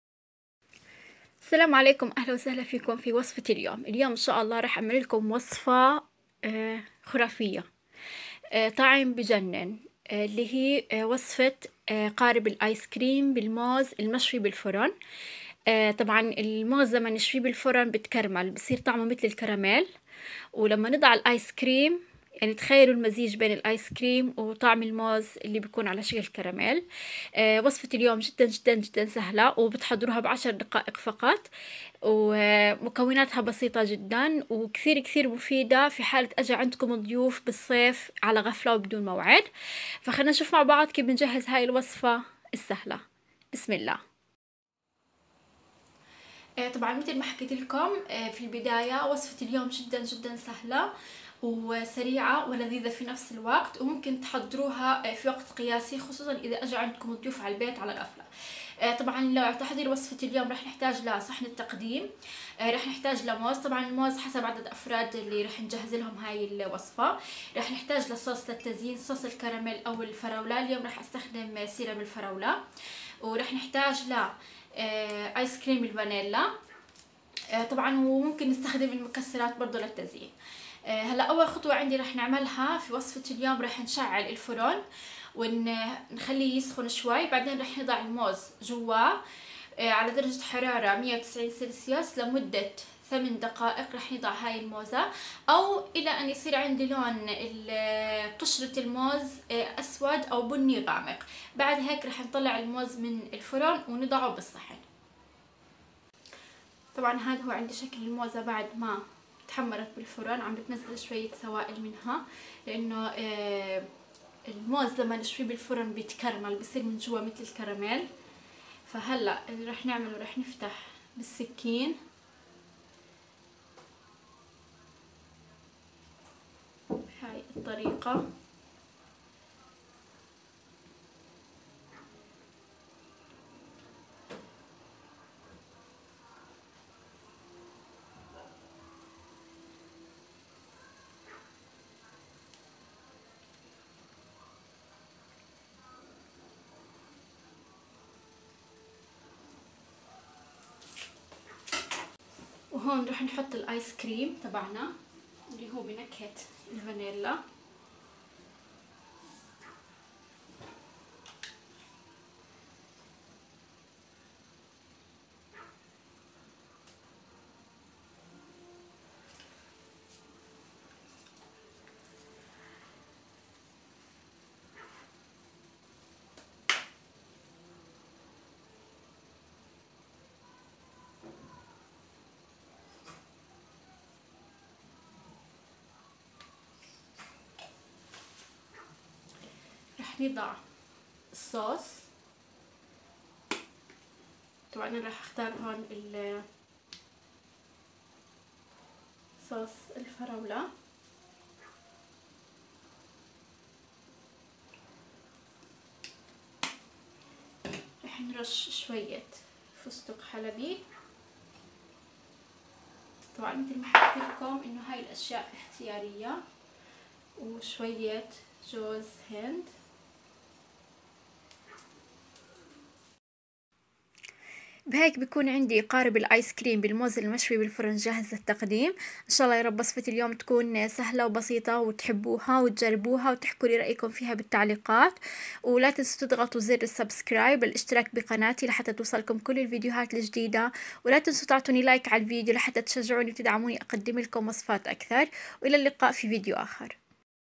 قارب الموز المشوي مع الايس كريم | قارب الايس كريم مع الموز في 10 دقائق | banana boat ice cream
يسعد مساكم يا حلوين .ز وصفتي لليوم خاصة و فقط للناس الرايقة والحلوة .. قارب الموز المشوي مع الايس كريم .. طعمها خرافي .. وسعلة التحضير في 10 دقائق فقط بتكون جاهزة.

قارب الموز مع الايس كريم ... قارب الموز .. قارب الايس كريم مع الموز.. وصفات الصيف.. حلا الصيف.. آيس كريم .. قارب الموز .. حلويات الصيف ..